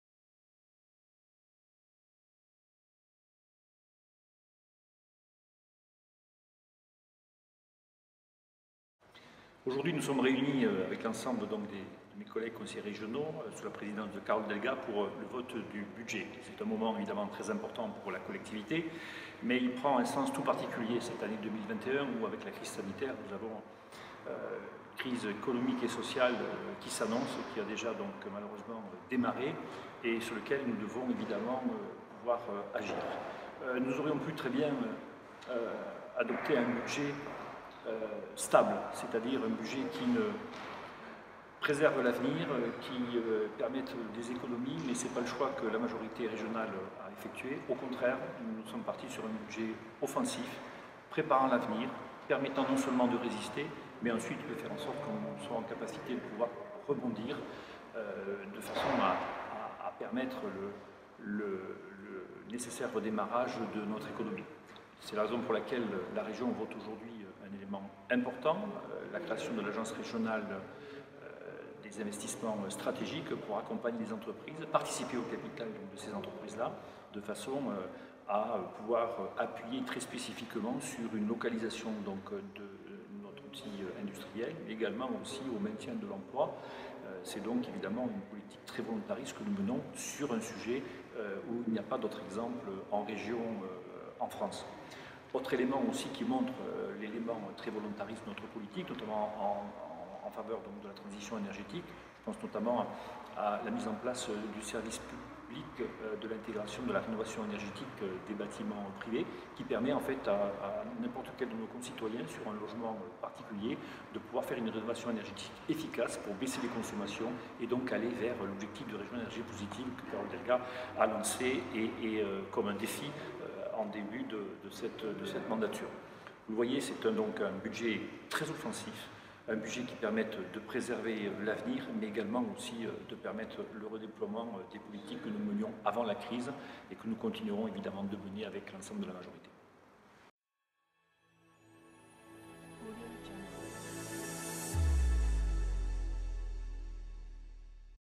Un budget 2021 très offensif en Occitanie
Thierry Cotelle, conseiller régionale d'Occitanie, évoque le vote du budget 2021 en Assemblée Plénière.